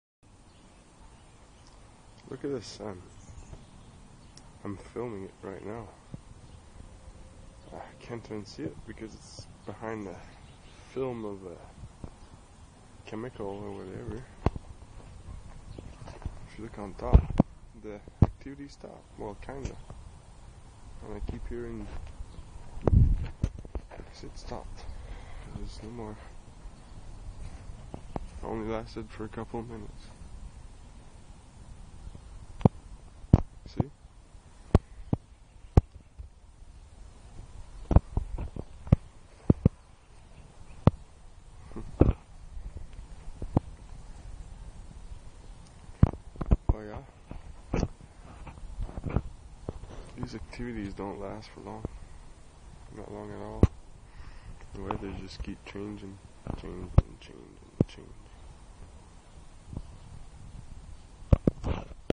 Sky after haarp
after haarp made the cloud go all crazy